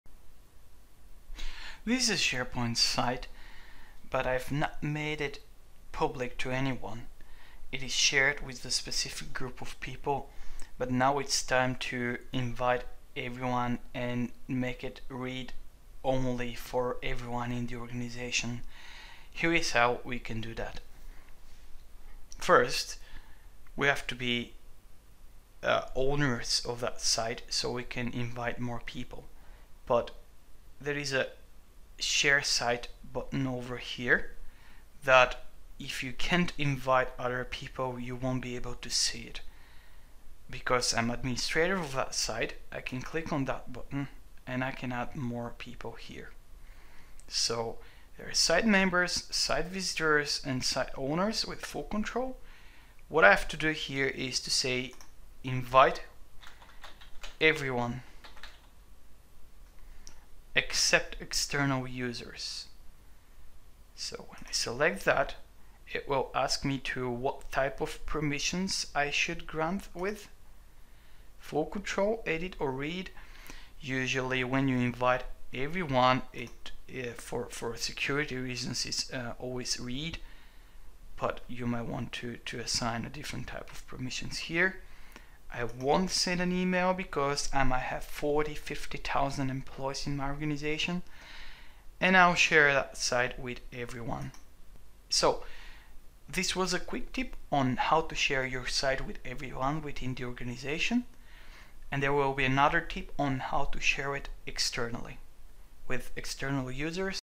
How to invite everyone to your SharePoint site
Many new SharePoint users have no clue how to make their site public to everyone in the organization. This is a quick video on how to do that.

Inviting more users to a site requires higher level of permissions to the SharePoint site and if you do not see some of the buttons form the video you should contact your SharePoint administrator.